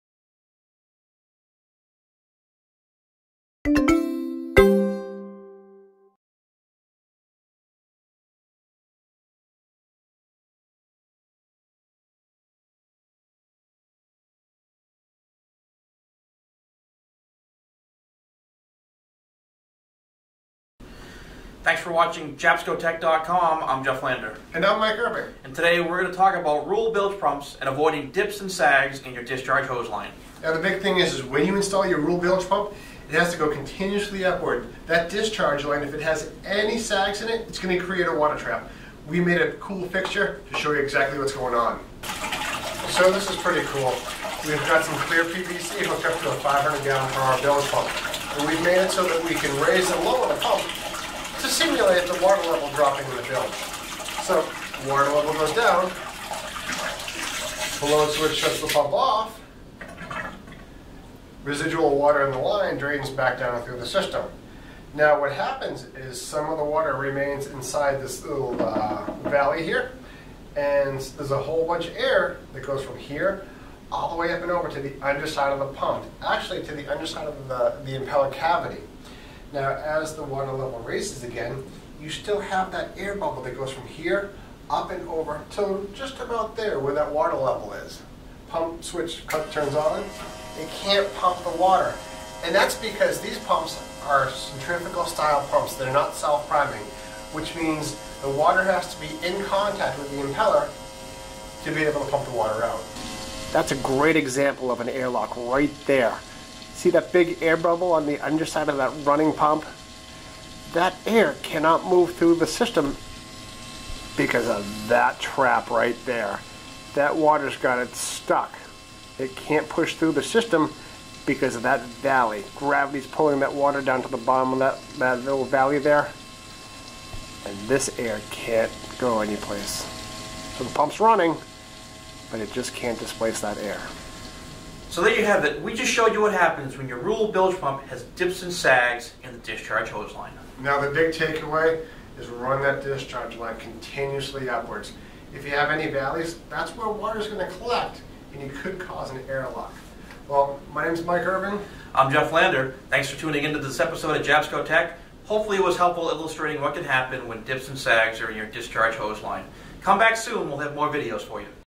Rule Bilge Pumps Explaining an Airlock in a Bilge Hose Video
- This video explains an airlock in a bilge pump hose.